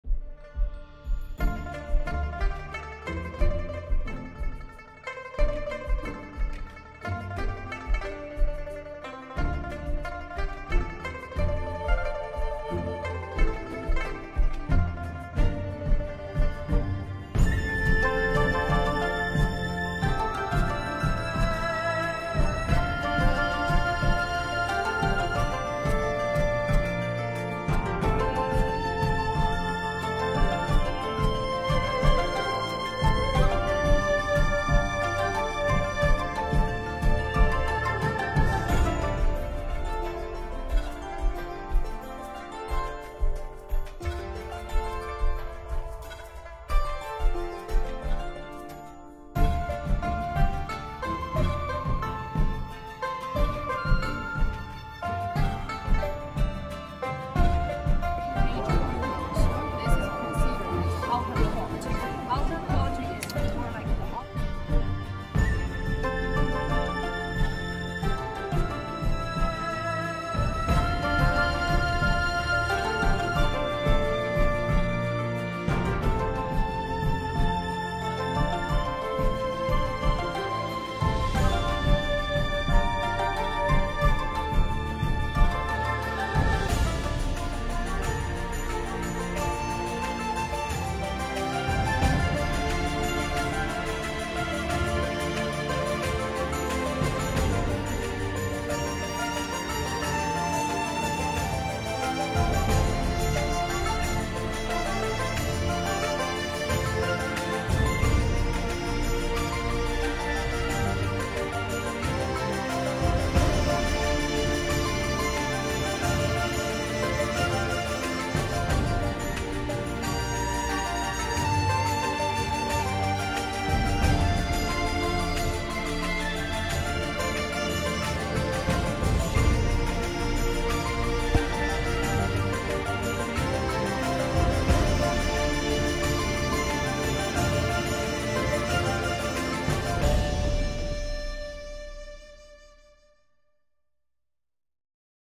Forbidden City
The Palace Museum is a national museum housed in the Forbidden City at the core of Beijing